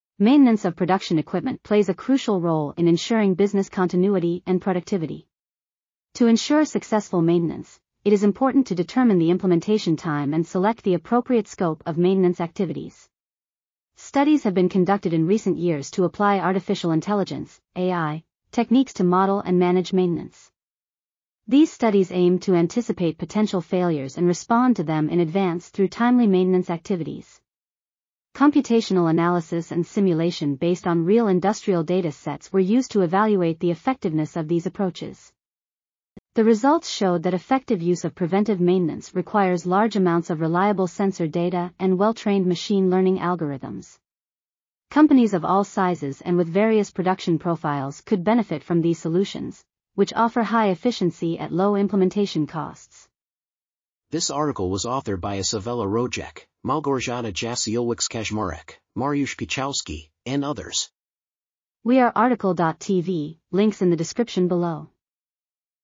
An Artificial Intelligence Approach for Improving Maintenance to Supervise Machine Fa... | RTCL.TV
### Keywords ###

### Article Attribution ###
Title: An Artificial Intelligence Approach for Improving Maintenance to Supervise Machine Failures and Support Their Repair
Authors: Izabela Rojek, Małgorzata Jasiulewicz-Kaczmarek, Mariusz Piechowski ,and Dariusz Mikołajewski

### Video Timestamps ###
0:00:00 - Summary
0:00:53 - Title
0:01:01 - Outro
0:01:05 - End